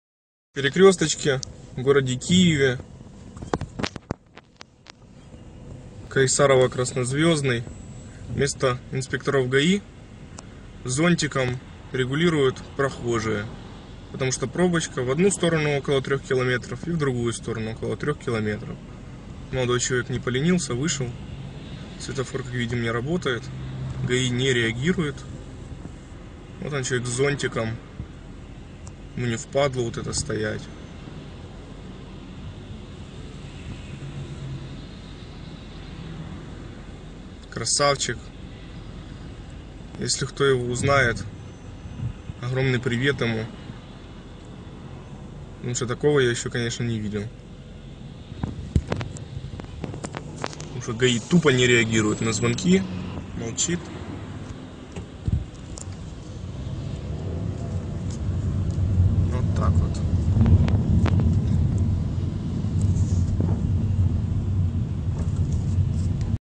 Good Samaritan with an umbrella regulates 6km traffic backup
While Ukrainian police is busy dealing with the protesters this citizen steps up.